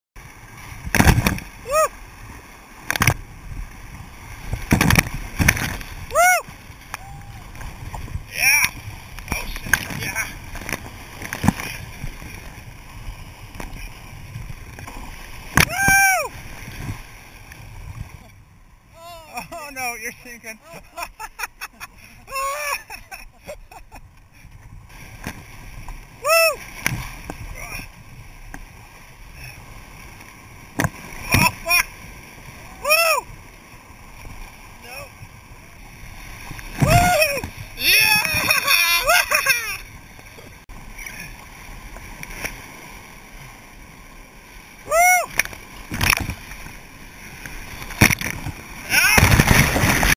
Lower Klickitat River kayaking highlights
Kayaking teaser #1: A few seconds of video from kayaking to tease the larger video I hope to post when I get back to my editing computer in Phoenix. This was all taken on the Klickitat near Lyle, Washington.